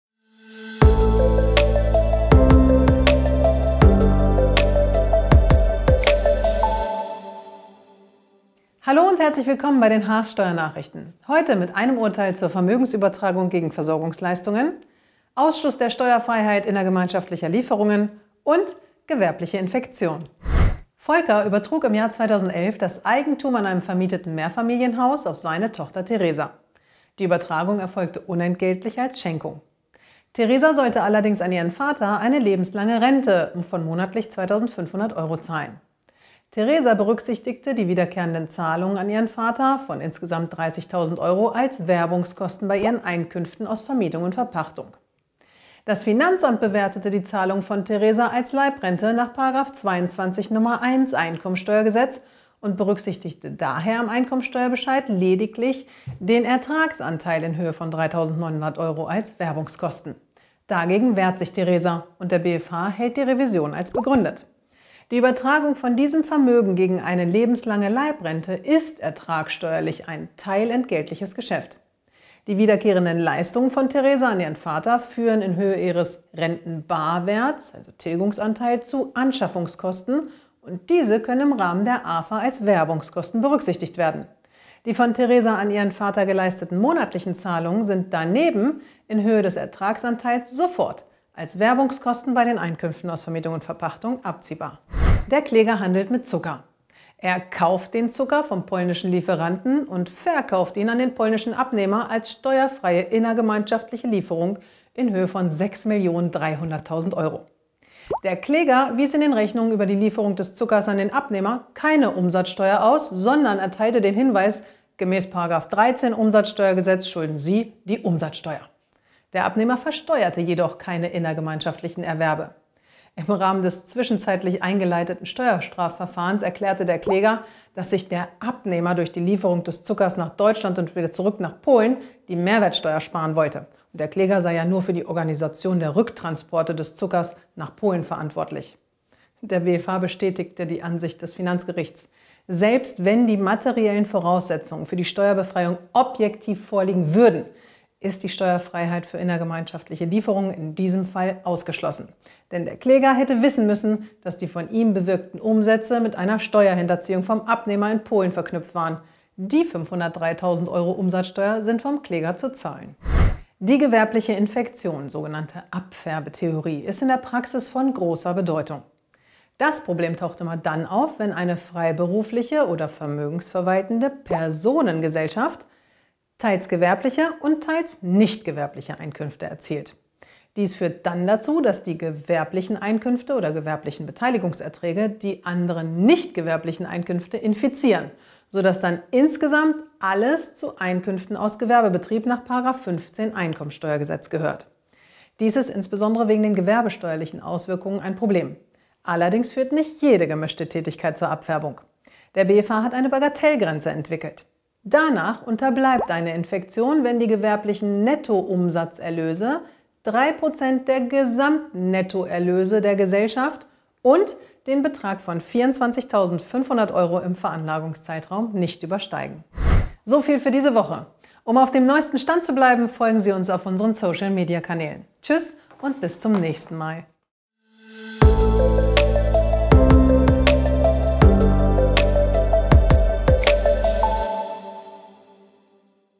Steuernachrichten - Update 06/2022 - Kostenlos & schnell informiert!
00:20 - Vermögensübertragung gegen Versorgungsleistungen
01:35 - Ausschluss der Steuerfreiheit innergemeinschaftlicher Lieferungen
02:43 - Gewerbliche Infektion